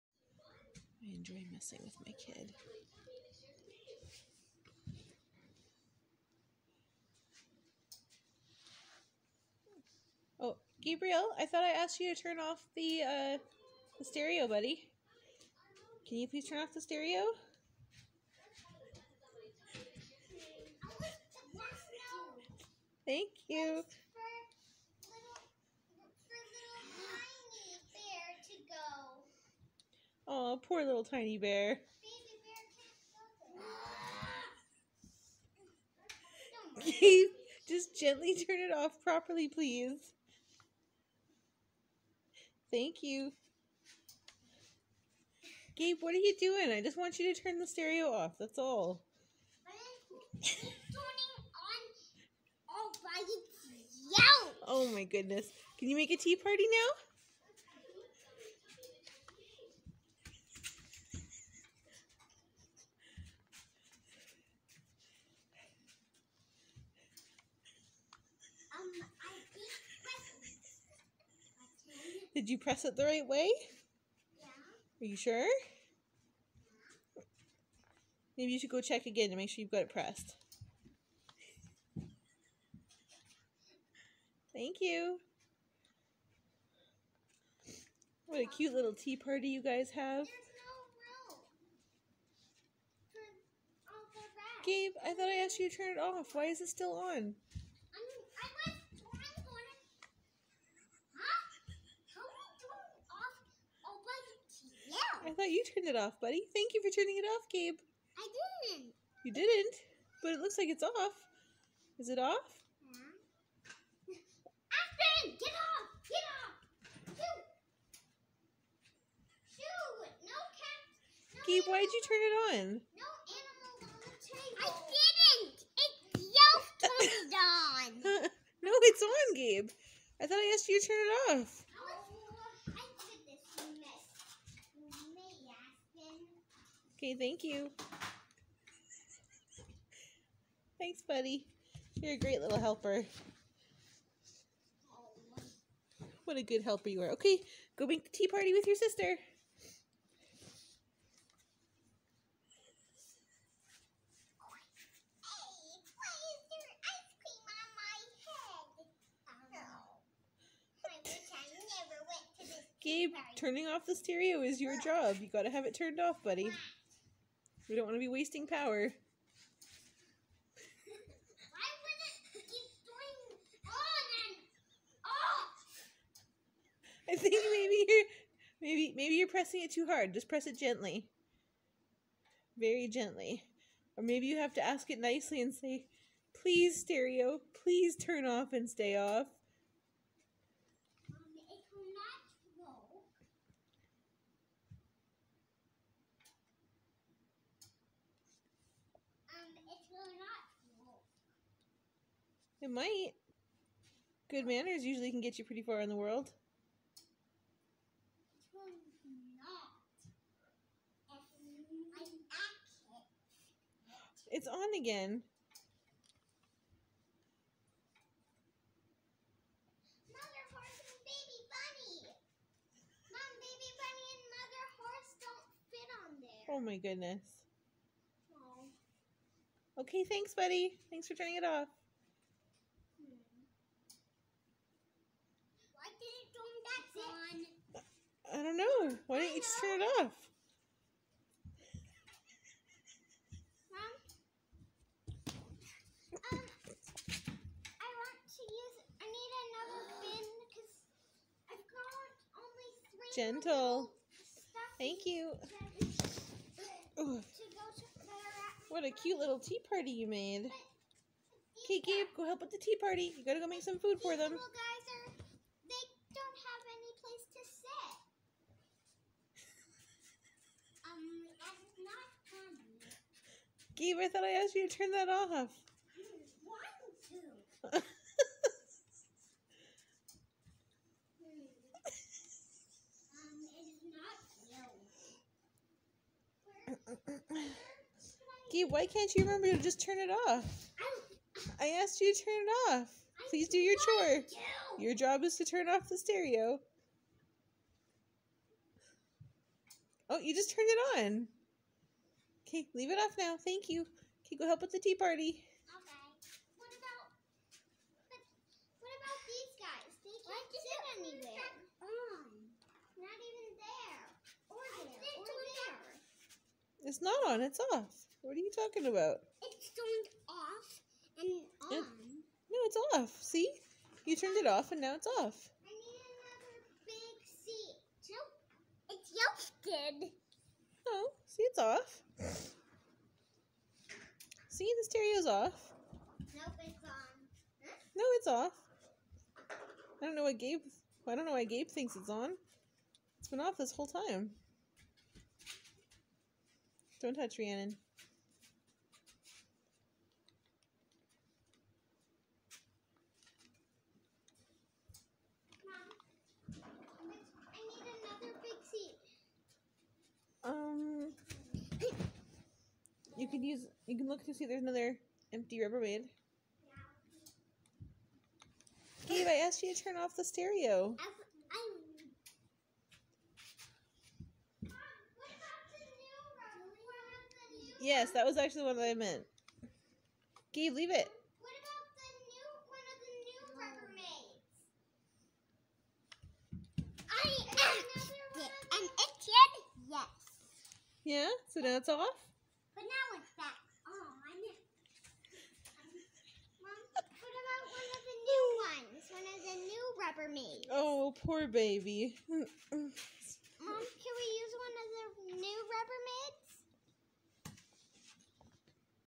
Pranking my 3 year old with Stereo Remote
I ask Gabriel to turn off the stereo, but I turn it back on again with the remote. 🤣